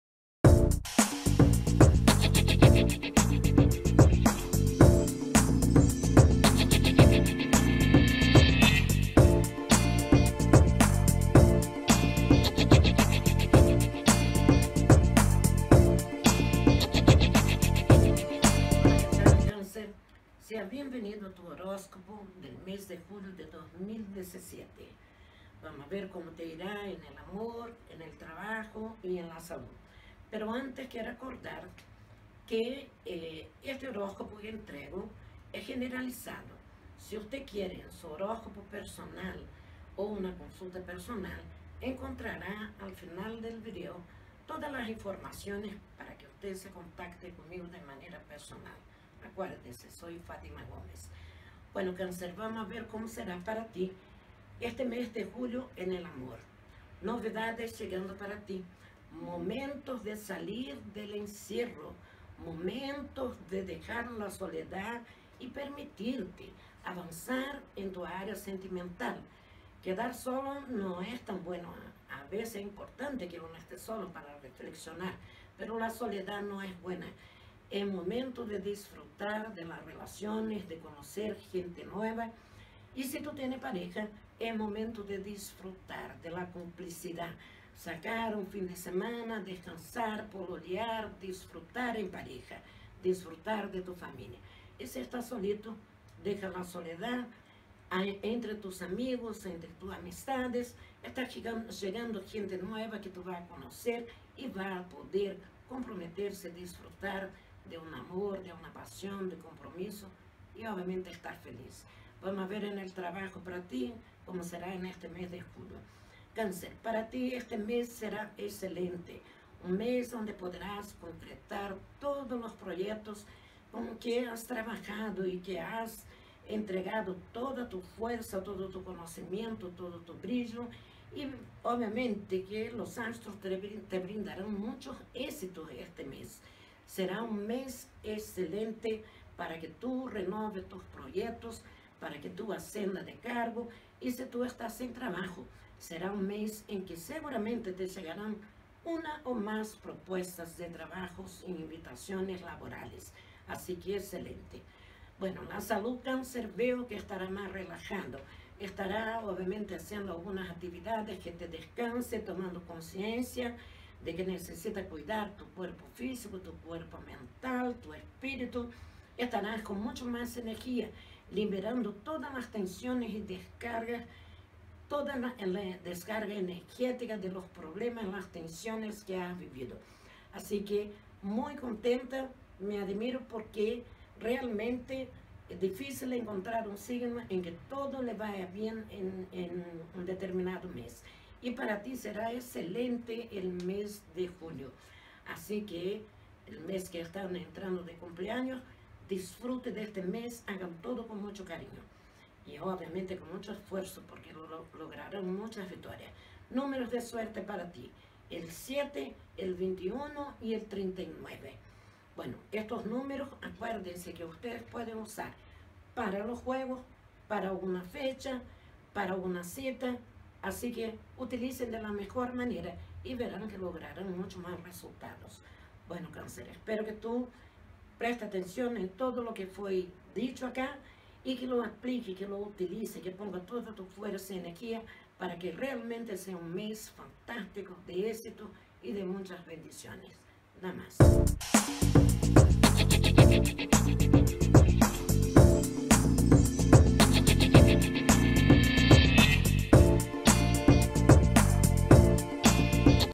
CANCER JULIO 2017
En este vídeo entregare el horóscopo de Julio para el signo de Cancer, hablare de amor, salud y trabajo. Entregando tips para que les valla super bien y puedan tener un excelente mes!!
¡Namaste!
Aplíquenlo!!!

Fatima Gomes Copyright ®.